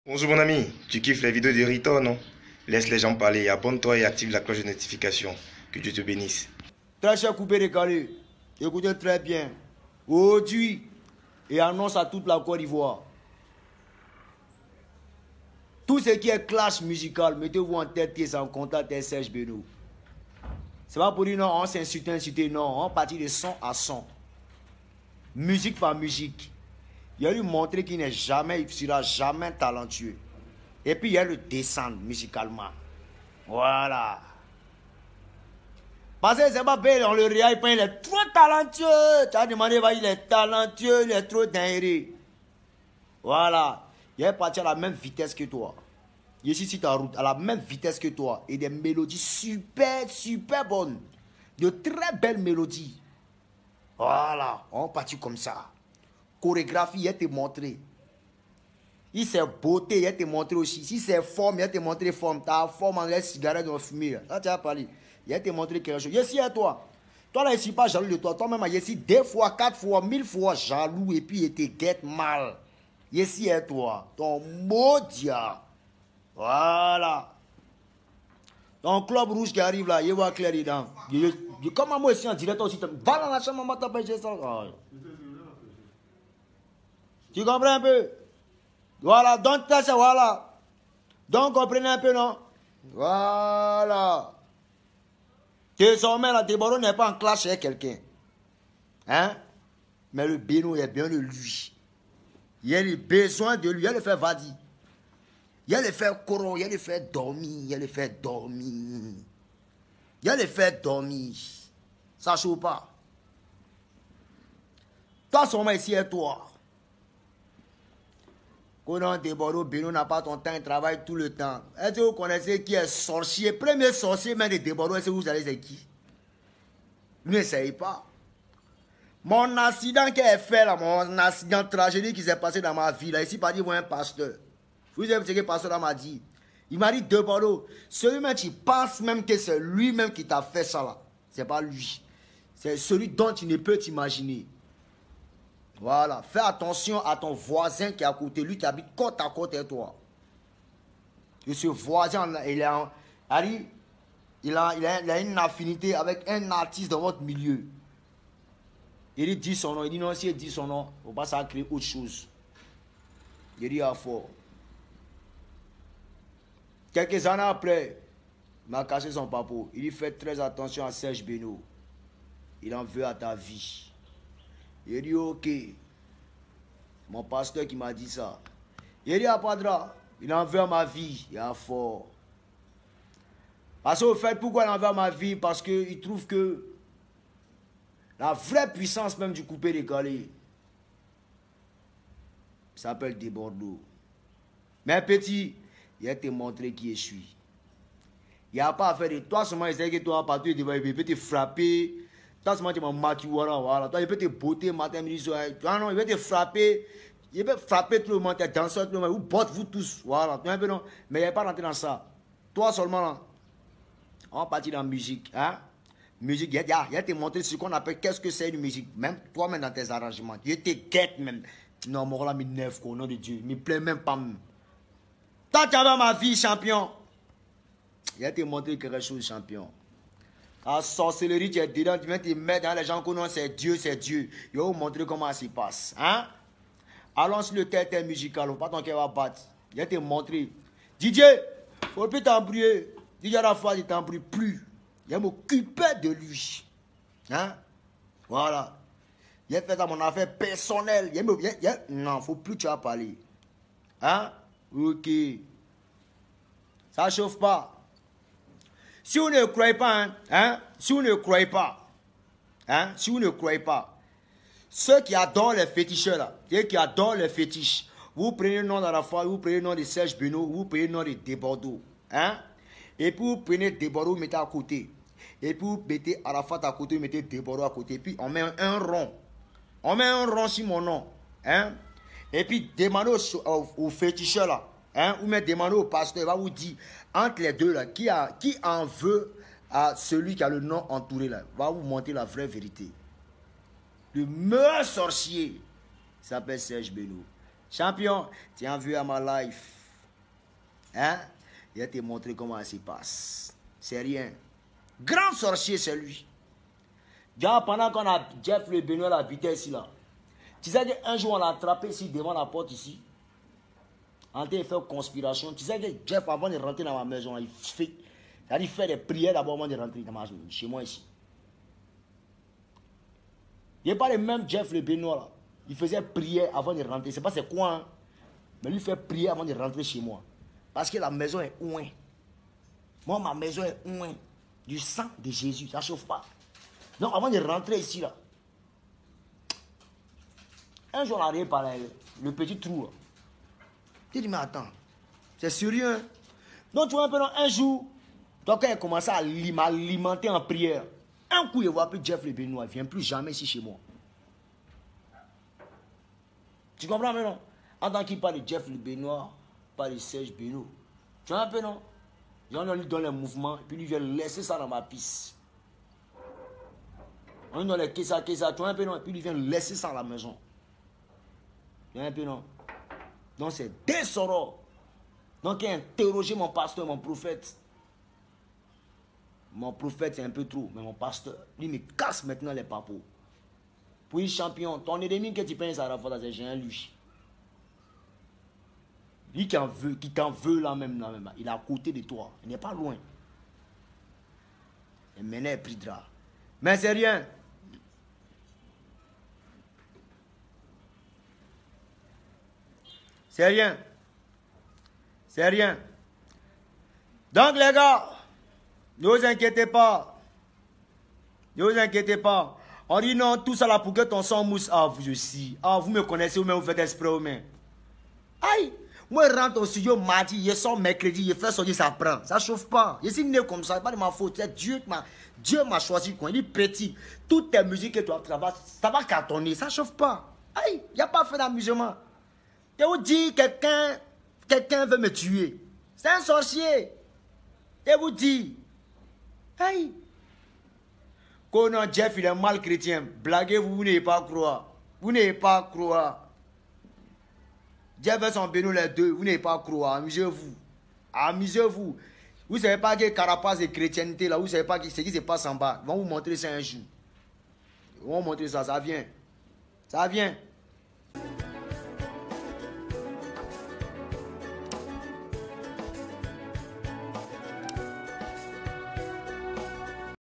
DEBORDO FAIT DE GRAVE RÉVÉLATIONS SUR SERGE BEYNAUD - IL EN VEUT À MA VIE
CONNAITRE SFM BÂCHE